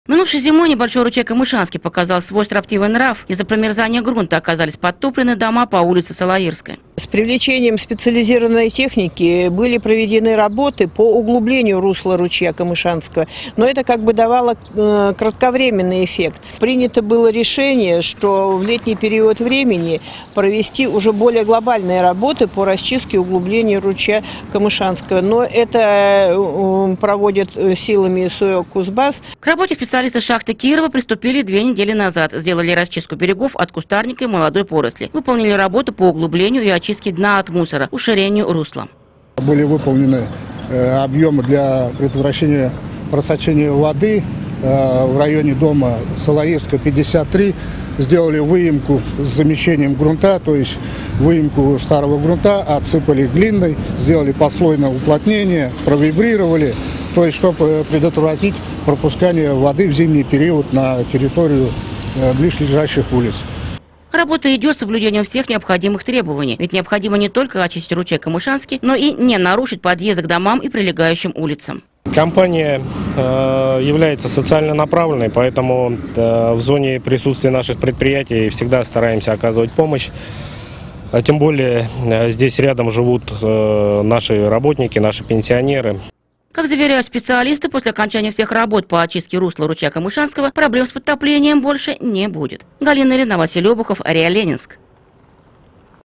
АО «СУЭК-Кузбасс» расчищает русло ручья Камышанский.
Шахта им. С.М. Кирова АО СУЭК-Кузбасс ведет работу по расчистке русла ручья Камышанский. Конечная цель - предотвратить угрозу подтопления близлежащих домов.